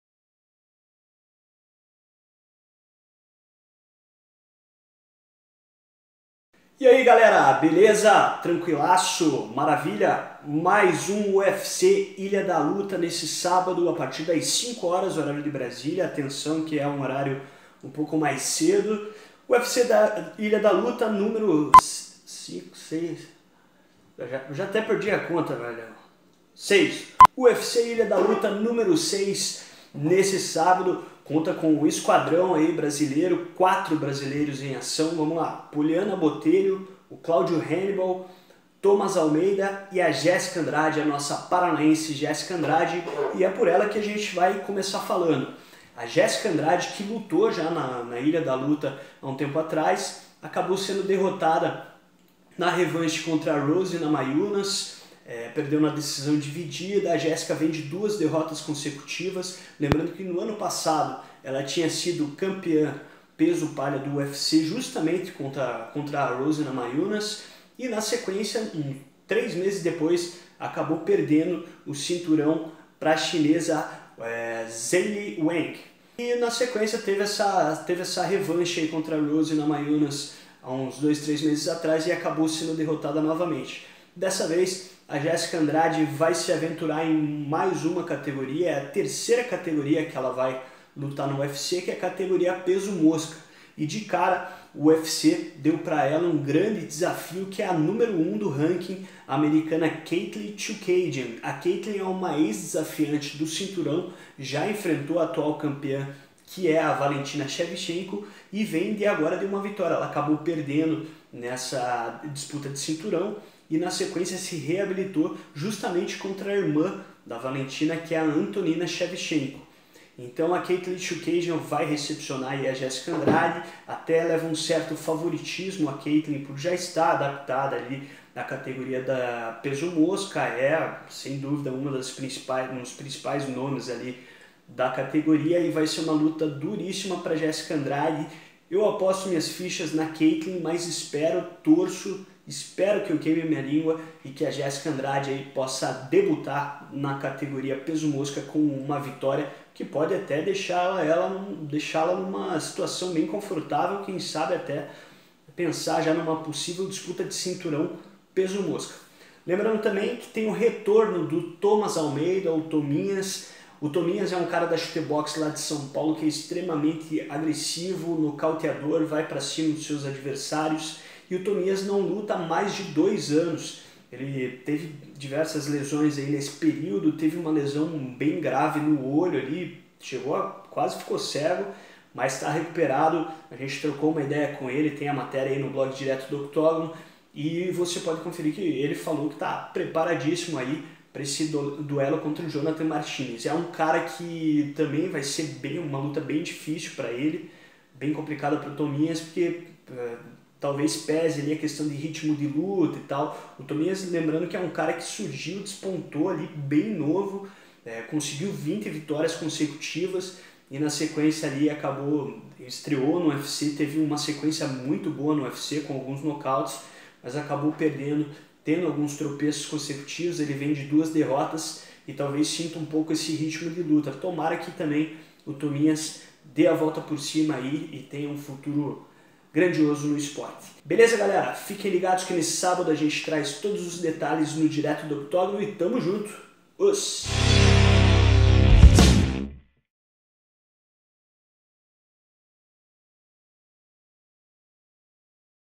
UFC Ilha da Luta 6 conta com quatro brasileiros
O card do UFC Ilha da Luta 6 ainda conta com a presença de quatro brasileiros: Jéssica Andrade, Thomas Almeida, Cláudio Hannibal e Poliana Botelho.

Confira o card completo, onde assistir o UFC Ilha da Luta 6 ao vivo e palpites